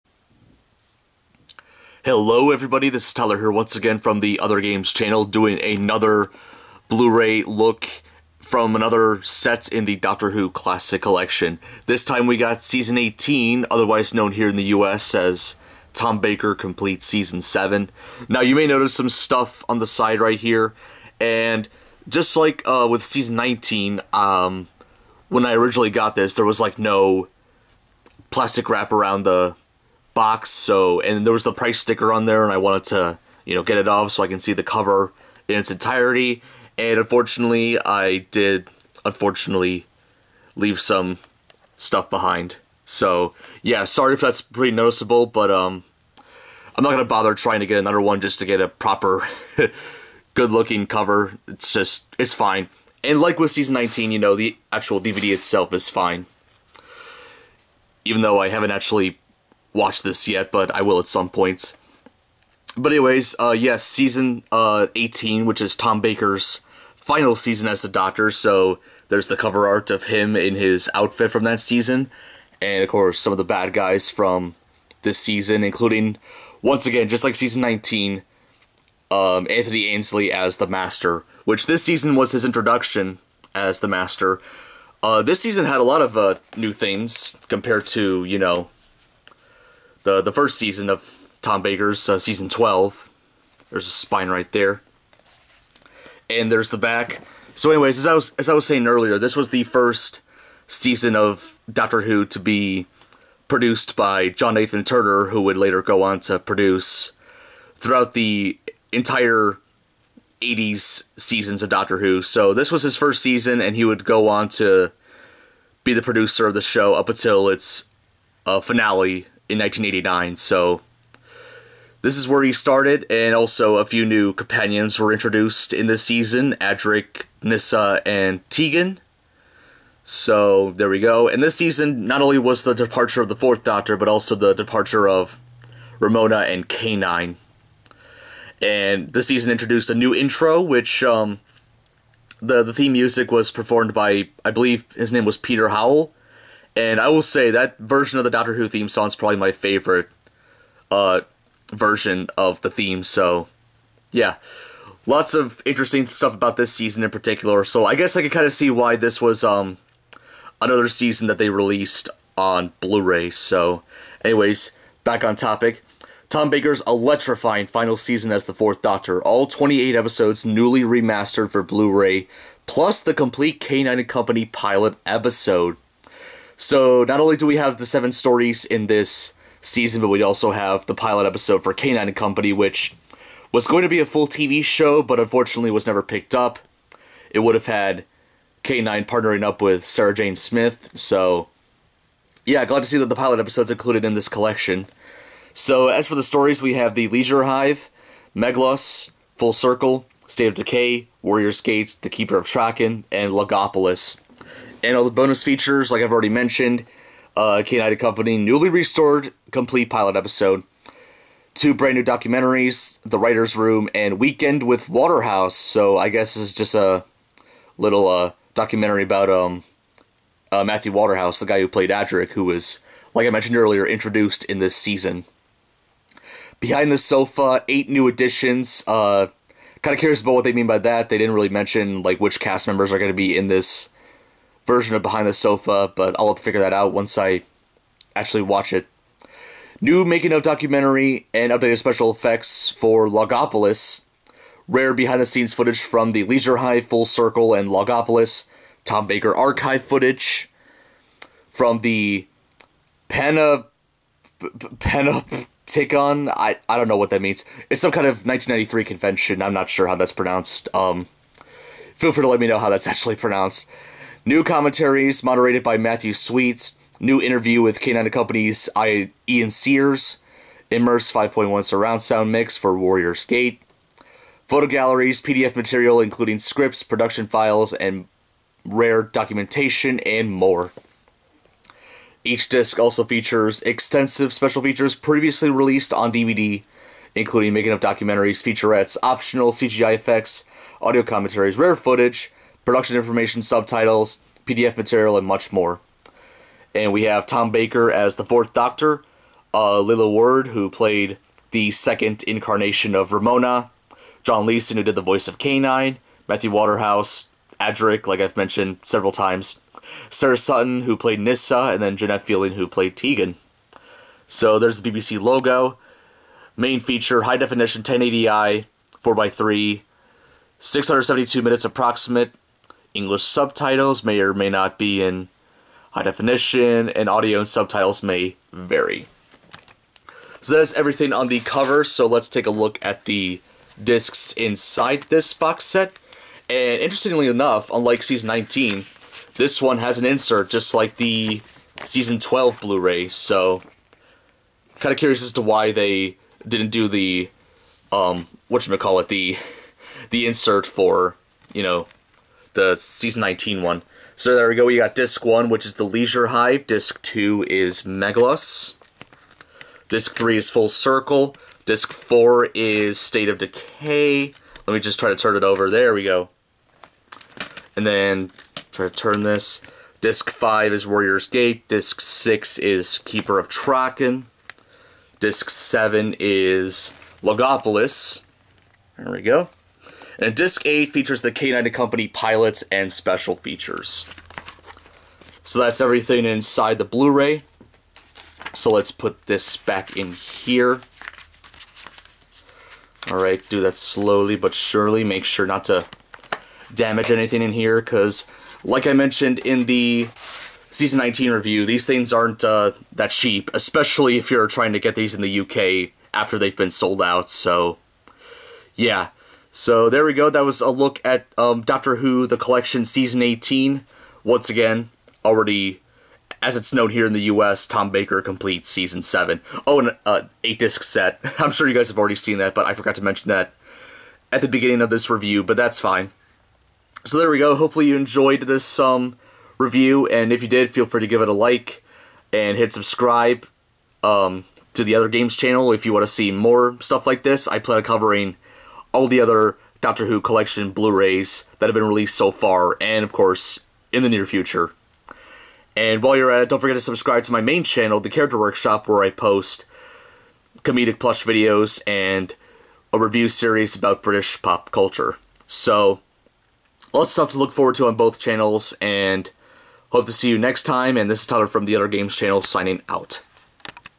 Doctor Who Season 18 (Tom Baker: Complete Season Seven) Blu-Ray
I take a look at the third season of classic Doctor Who to be released on blu-ray. Season 18 (Tom Baker's seventh and final season)